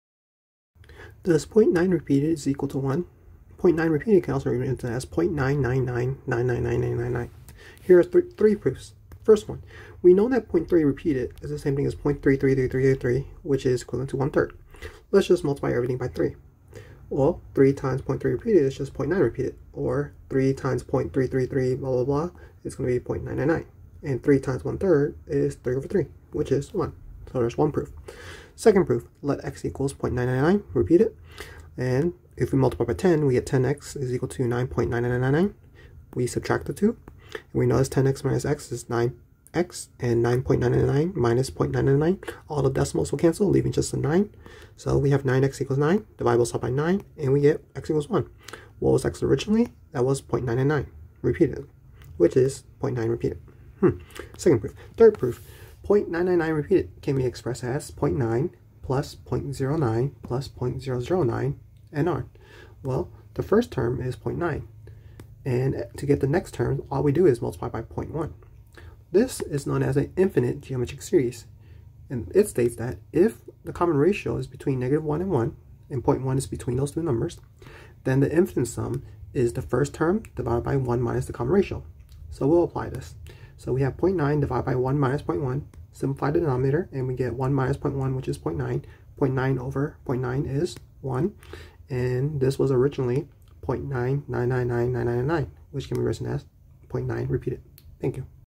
Does .999...=1? Explained
Does .999...=1?

We will show three proofs that this is indeed true.